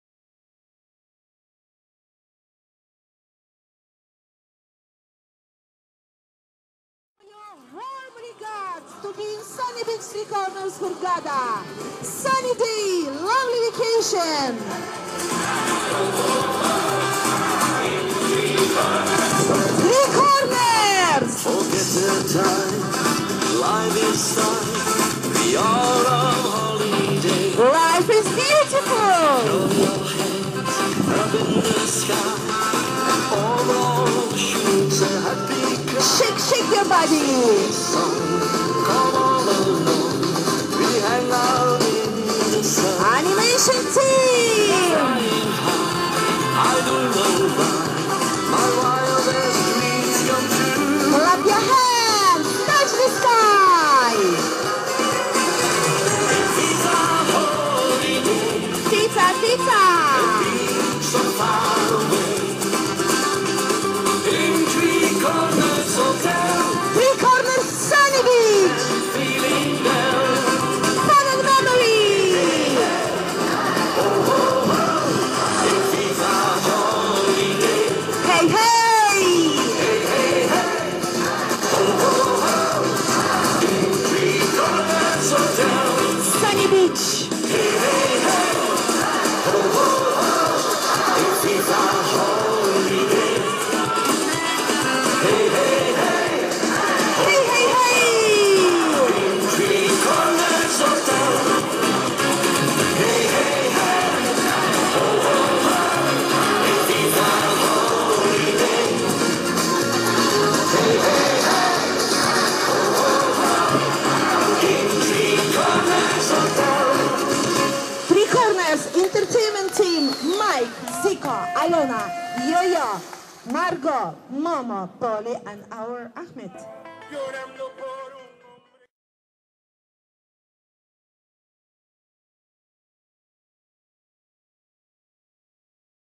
THE THREE CORNERS HOTEL, SUNNY BEACH, HURGHADA , CLUB SONG MAY 2013 by Mieke Aerts
Club dance and song THE THREE CORNERS HOTEL, SUNNY BEACH , HURGHADA MAY 2013